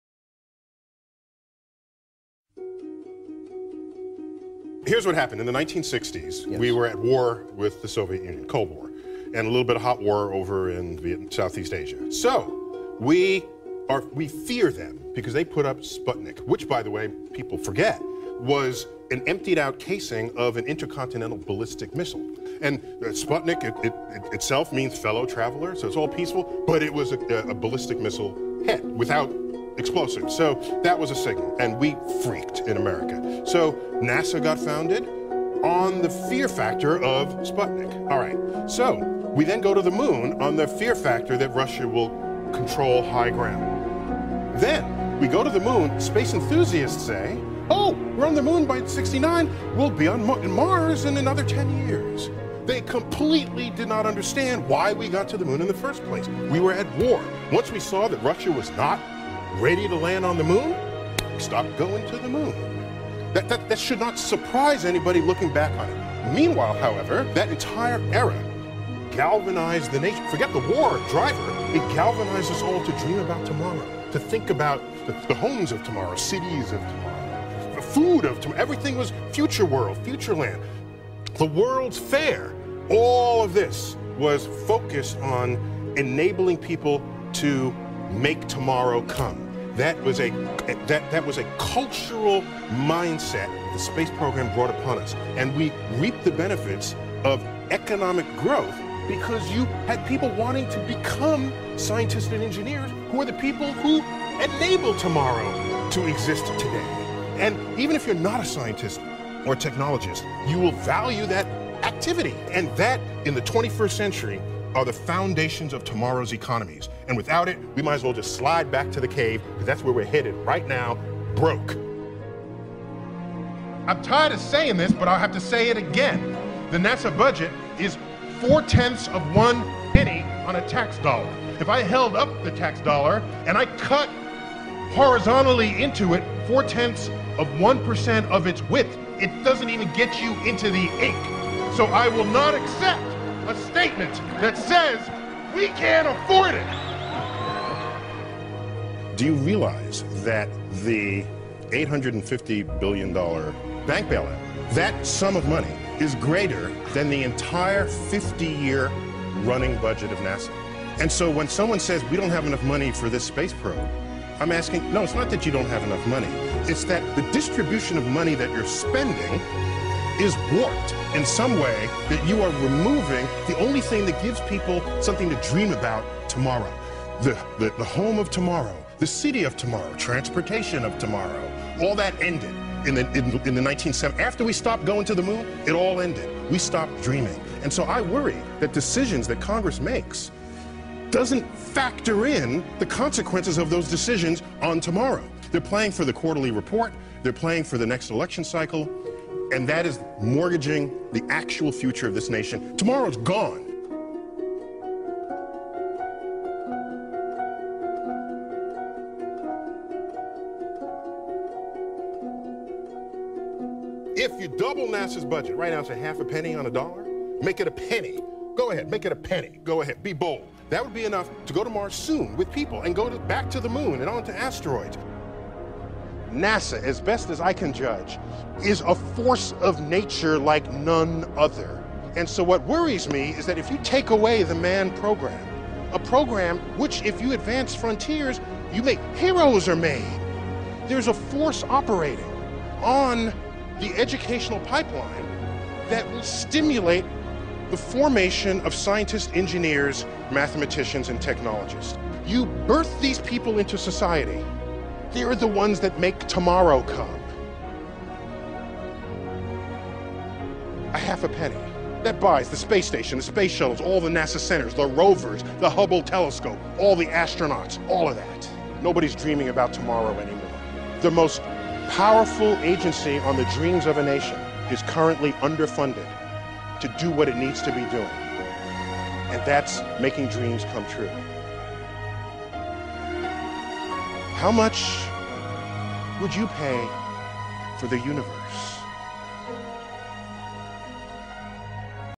Neil deGrasse Tyson - We Stopped Dreaming
Here's what happened. In the 1960's we were at war with the Soviet Union - Cold War. And a little bit of hot war over in Southeast Asia (Vietnam).

We feared them because they put up Sputnik.

So, we feared them [Soviet Union] because they put up Sputnik. Which, by the way, people forget was an emptied out casing of an intercontinental ballistic missile. Sputnik itself means "Fellow Traveler". So, it sounds all peaceful, but it was a ballistic missile head without explosives.

So, that was a signal and we freaked in America. So NASA got founded on the fear factor of Sputnik. Alright, so we then go to the Moon on the fear factor that Russia will control high ground.

Then we go the moon. Space enthusiasts say, "Oh, we're on the Moon by '69! We'll be on Mars in another 10 years." They completely did not understand why we got to the Moon in the first place - we were at war. Once we saw that Russia was not ready to land on the Moon, we stopped going to the Moon. That should not surprise anybody looking back on it.

That entire era galvanized the nation.

Meanwhile, however, that entire era galvanized the nation. Forget the war driver, it galvanized us all to dream about tomorrow. To think about the homes of tomorrow. The cities of tomorrow. The food of tomorrow. Everything was future world - future land.

The World's Fair - all of this - was focused on enabling people to make tomorrow come. That was a cultural mindset the space program brought upon us. And we reaped the benefits of economic growth because you had people wanting to become scientists and engineers - who are the people who enable tomorrow to exist today.

And even if you're not a scientist or technologist, you will value that activity. That, in the 21st Century, are the foundation of tomorrow's economies and without it, we might as well just slide back to the caves because that's where we're heading right now - broke.

I will not accept a statement that says we cannot afford it.

I'm tired of saying this, but I'll have to say it again, "The NASA budget is four-tenths of one penny on a tax dollar." If I held up the tax dollar and I cut - horizontally into it - four tenths of one percent of it's width, it doesn't even get you into the ink. So I WILL NOT ACCEPT A STATEMENT THAT SAYS, "We can't afford it."

Do you realize that the $850 billion dollar bank bailout - that sum of money is greater than the entire 50 year running budget of NASA? And so, when someone says, "We don't have enough money for this space probe." I'm asking, "No. It's not that you don't have enough money." It's that the distribution of money that you're spending is warped in some way that you are removing the only thing that gives people something to dream about tomorrow.

The home of tomorrow. The city of tomorrow. The transportation of tomorrow. All that ended in the 1970's. After we stopped going to the Moon, it all ended - We stopped dreaming.

And so I worry, that decisions that Congress makes doesn't factor in the consequences of those decisions on tomorrow. They're playing for the quarterly report. They're playing for the next election cycle. And that is mortgaging the actual future of this nation. Tomorrow is gone.

NASA is a force of nature like none other.

If you double NASA's budget - right now it's a half-a-penny on a dollar - make it a penny. Go ahead - make it a penny. Go ahead - be bold! That would be enough to go to Mars soon - with people. And go back to the Moon and on to asteroids.

NASA, as best as I can judge, is a force of nature like none other. And so what worries me is that if you take away the manned program. A program which, if you advance frontiers, heroes are made.

There is a force operating, on the educational pipeline, that will stimulate the formation of scientists, engineers, mathematicians and technologists. You birth these people into society. They are the ones that make tomorrow come.

A half a penny.

That buys the space station, the space shuttles, all the NASA centers, the rovers, the Hubble telescope, all the astronauts, all of that.

Nobody's dreaming about tomorrow anymore. The most powerful agency on the Dreams of a Nation is currently underfunded to do what it needs to be doing and that's making dreams come true.

How much would you pay for the Universe?